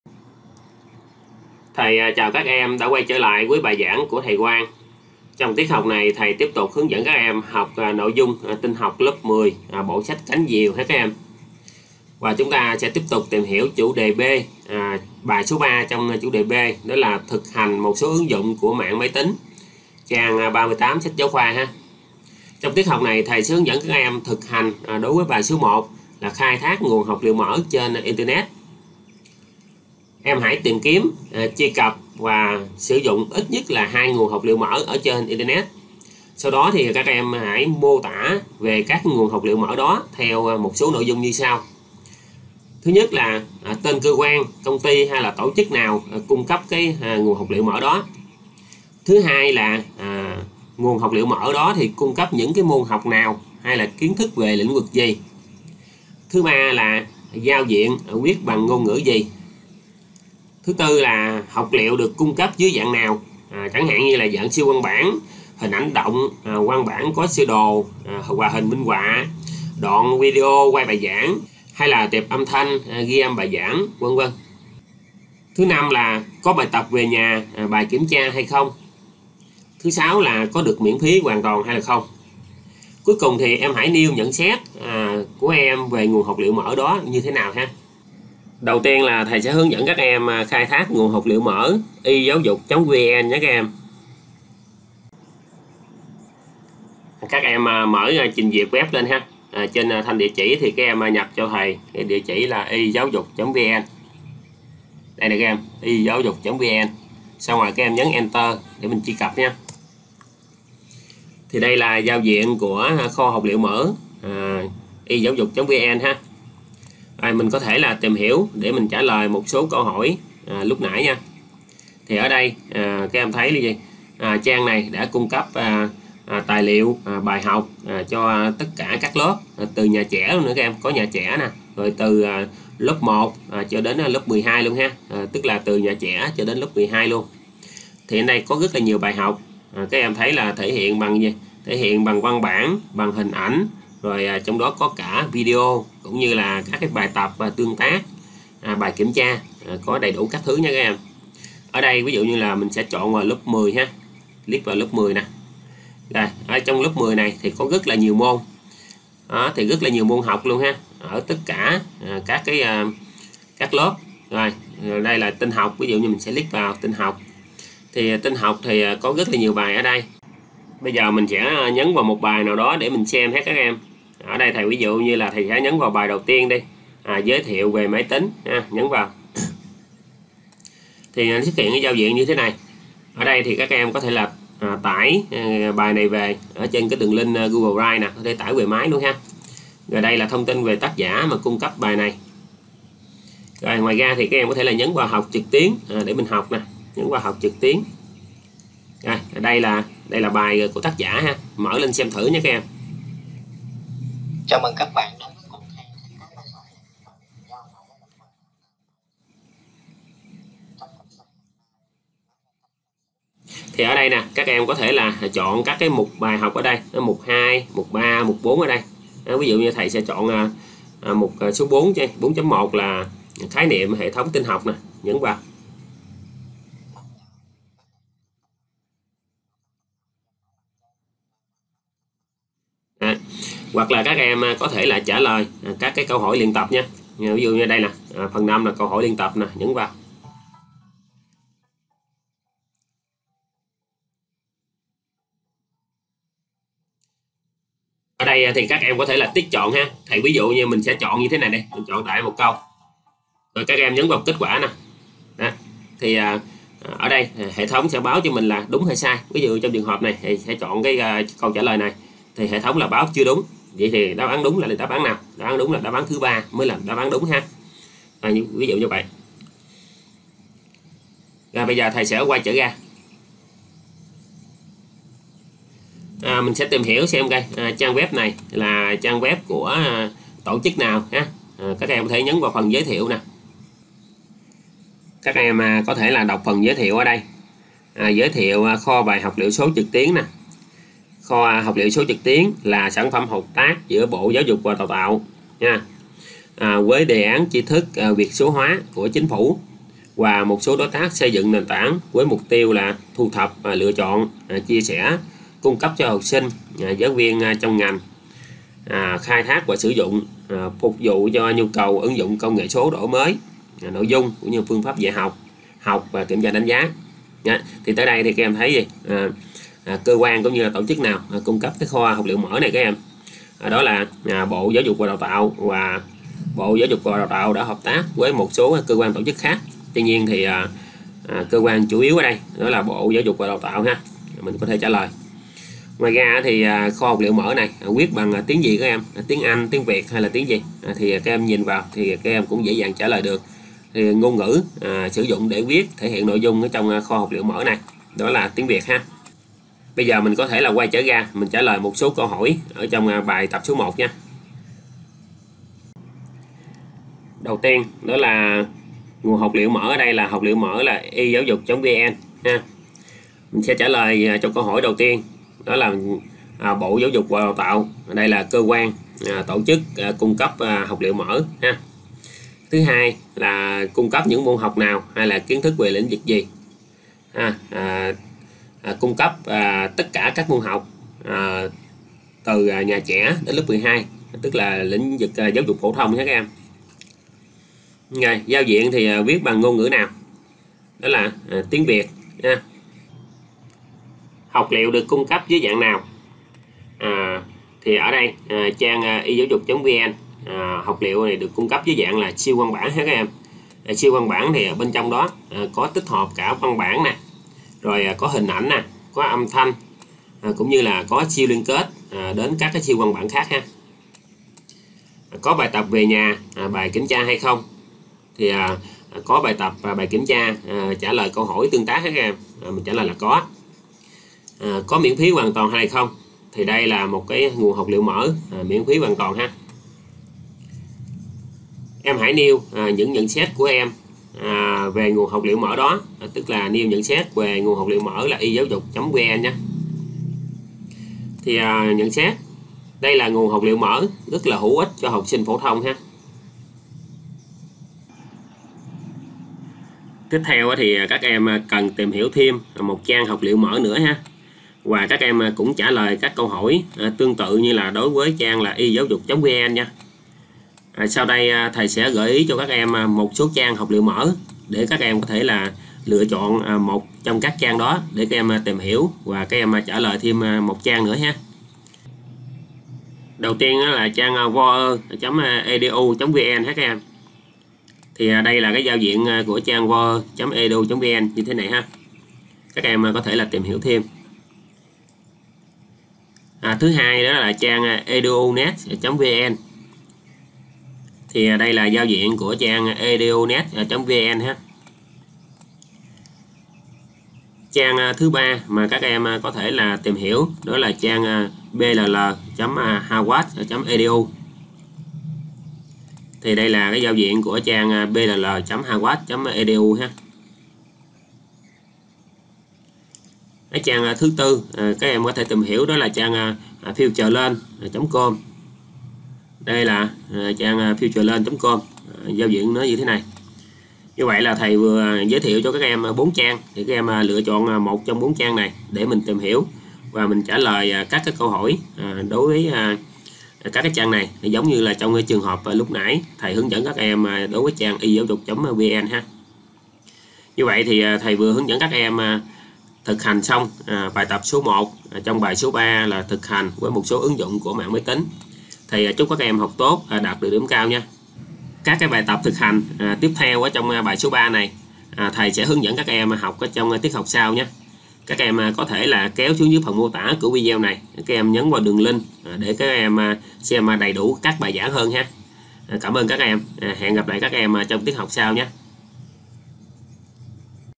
[Thực hành - Bài tập 1] - Bài 3 Chủ đề B Tin học 10 - Thực hành một số ứng dụng của mạng máy tính
Hướng dẫn làm thực hành, bài tập 1(Khai thác nguồn học liệu mở trên Internet, trang 38), bài 3 Thực hành một số ứng dụng của mạng máy tính, tin học lớp 10, Chủ đề B Mạng máy tính và Internet (Internet hôm nay và ngày mai), sách giáo khoa Cánh diều
Danh sách phát "Tin học 10 - Cánh diều", xem tất cả bài giảng tin học 10
Chúc các em học tập tốt và đạt điểm cao!